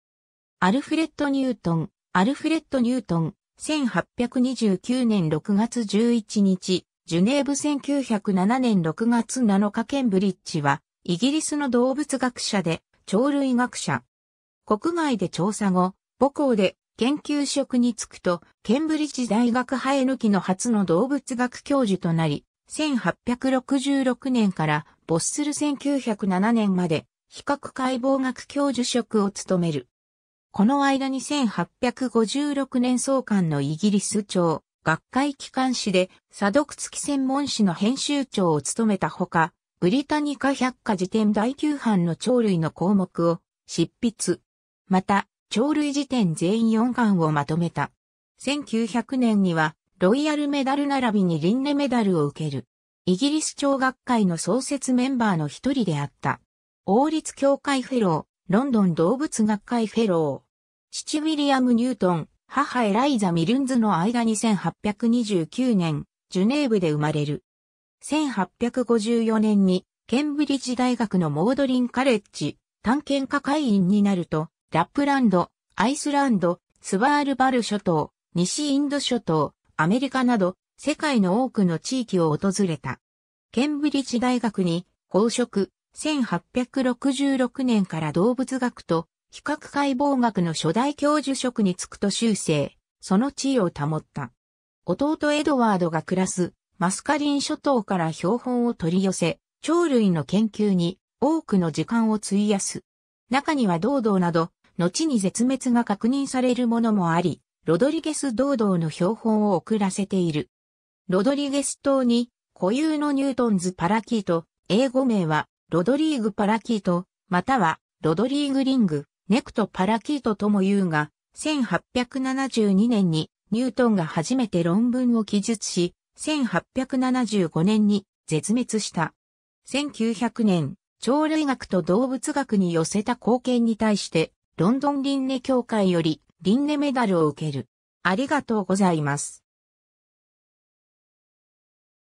アルフレッド・ニュートン
リンネ・メダル受賞者
アルフレッド・ニュートン (Alfred Newton, 1829年6月11日 ジュネーブ – 1907年6月7日 ケンブリッジ）は、イギリスの動物学者で鳥類学者。
国外で調査後、母校で研究職に就くと、ケンブリッジ大学はえぬきの初の動物学教授となり、1866年から没する1907年まで比較解剖学教授職を務める。
 この間に1856年創刊のイギリス鳥学会機関誌で査読付き専門誌の編集長（Ibis 1865年–1870年）を務めたほか、『ブリタニカ百科事典』第9版（1875年 - 1889年全24巻）の鳥類の項目を執筆。
また『鳥類事典』全4巻（Dictionary of Birds 1893年–1896年）をまとめた。
 1900年にはロイヤル・メダル（王立協会）ならびにリンネ・メダル（リンネ協会）を受ける。
イギリス鳥学会の創設メンバーのひとりであった。
王立協会フェロー（FRS、HFRSE）、ロンドン動物学会フェロー。
 父ウィリアム・ニュートン、母エライザ・ミルンズの間に1829年、ジュネーブで生まれる。
1854年にケンブリッジ大学のモードリン・カレッジ「探検家会員」になるとラップランド、アイスランド、スバールバル諸島、西インド諸島、アメリカなど世界の多くの地域を訪れた。
ケンブリッジ大学に奉職、1866年から動物学と比較解剖学の初代教授職につくと終生、その地位を保った。
 弟エドワードが暮らすマスカリン諸島から標本を取り寄せ、鳥類の研究に多くの時間を費やす。
中にはドードーなど、のちに絶滅が確認されるものもあり、ロドリゲスドードー （Pezophaps solitaria）の標本を送らせている。
ロドリゲス島に固有の Newton's parakeet（Psittacula exsul ）、英語名はRodrigues parakeet または Rodrigues ring-necked parakeet ともいうが、1872年にニュートンが初めて論文を記述し、1875年に絶滅した。
 1900年、鳥類学と動物学に寄せた貢献に対してロンドン・リンネ協会よりリンネ・メダルを受ける。